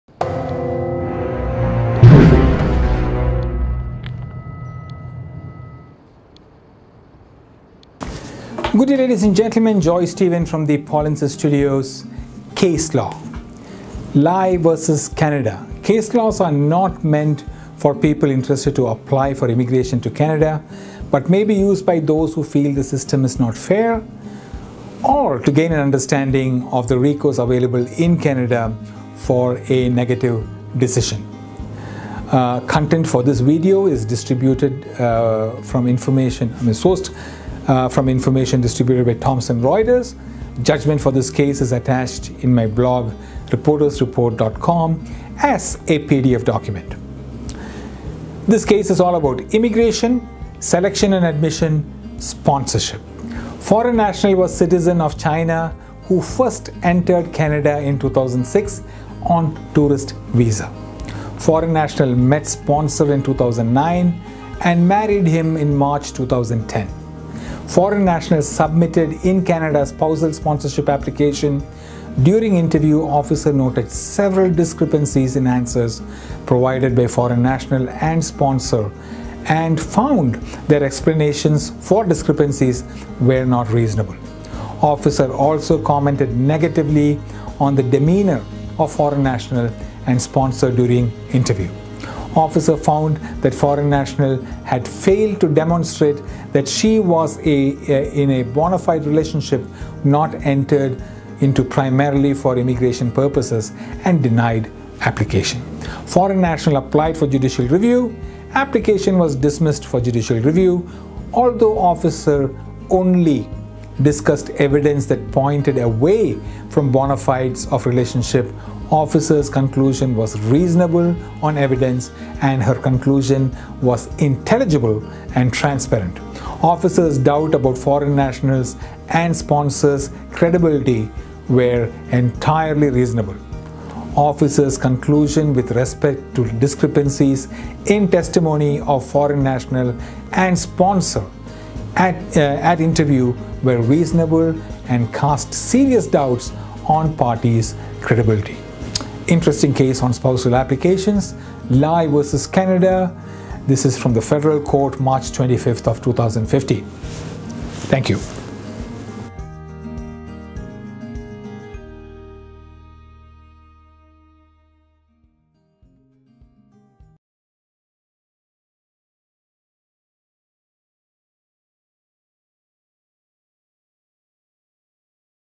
Canada Immigration Case-law Canada v Lai, IMMIGRATION — Selection and admission — Sponsorship
Foreign national was citizen of China who first entered Canada in 2006 on tourist visa — Foreign national met sponsor in 2009 and married him in March 2010 — Foreign national submitted in-Canada spousal sponsorship application — During interview officer noted several discrepancies in answers provided by foreign national and sponsor, and found their explanations for discrepancies were not reasonable — Officer also commented negatively on demeanor of foreign national and sponsor during interview — Officer found that foreign national had failed to demonstrate that she was in bona fide relationship not entered into primarily for immigration purposes and denied application — Foreign national applied for judicial review — Application dismissed — Although officer only discussed evidence that pointed away from bona fides of relationship, officer’s conclusions were reasonable on evidence, and her reasons were intelligible and transparent — Officer’s doubts about foreign national's and sponsor’s credibility were entirely reasonable — Officer’s conclusions with respect to discrepancies in testimony of foreign national and sponsor at interview were reasonable and cast serious doubts on parties' credibility